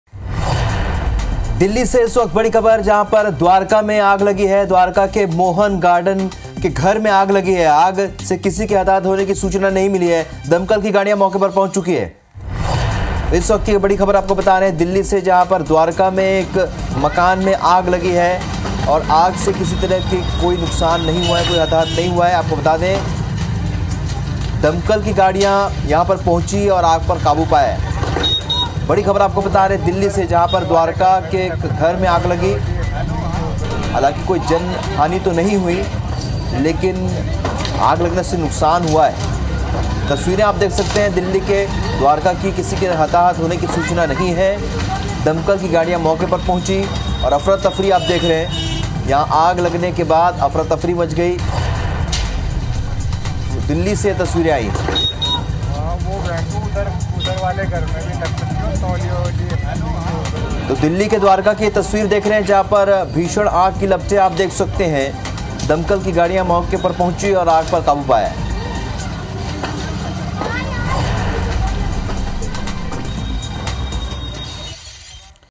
Delhi Fire : द्वारका के मोहन गार्डन इलाके में लगी भीषण आग | दमकल की 4 गाड़ियां मौके पर
IBC24, Delhi Fire, द्वारका के मोहन गार्डन इलाके में लगी भीषण आग, दमकल की 4 गाड़ियां मौके पर